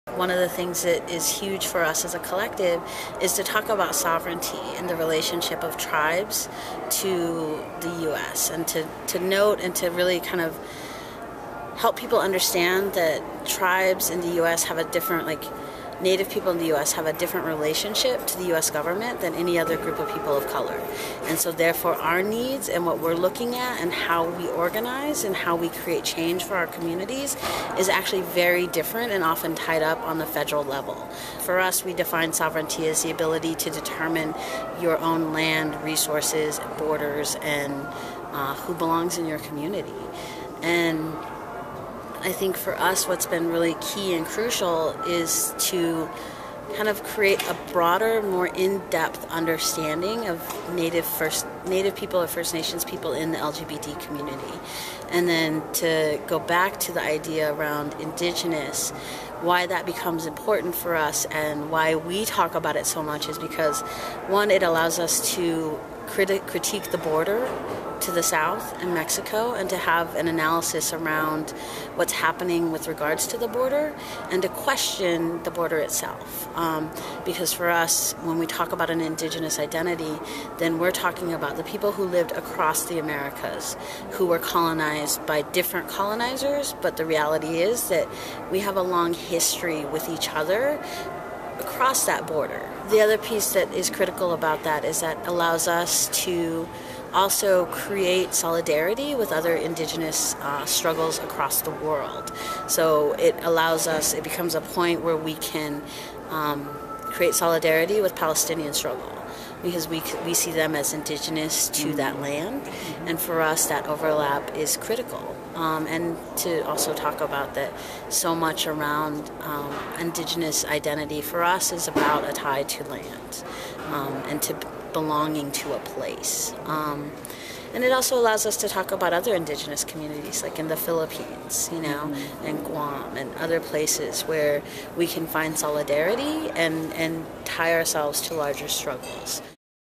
First Nations Two Spirit Collective: Coya
While at the BOLD Gathering in Minneapolis in December 2011, Astraea caught up with Coya, a member of the First Nations Two Spirit Collective. Coya tells us about the collective's work to bring the issue sovereignty and indigenous identity front and center in the LGBT movement. The First Nations Two Spirit Collective is a newer organization funded by Astraea through the U.S. General Fund. The Collective is comprised of Indigenous peoples who identify as two spirit, gay, lesbian, bisexual, transgender, gender-non-conforming, queer and allies. Of the 400 Native cultures on Turtle Island (what is now considered North America), over 155 have documented traditions of Two Spirit members. Colonization, genocide and religious persecution have all played a part in making the history of Two Spirit people invisible. Today, Two Spirit teens have the highest suicide rate of all ethic groups. The Collective addresses issues of Two Spirit people through organizing and education and by promoting personal and spiritual sustainability in the larger social justice and LGBTQ movement.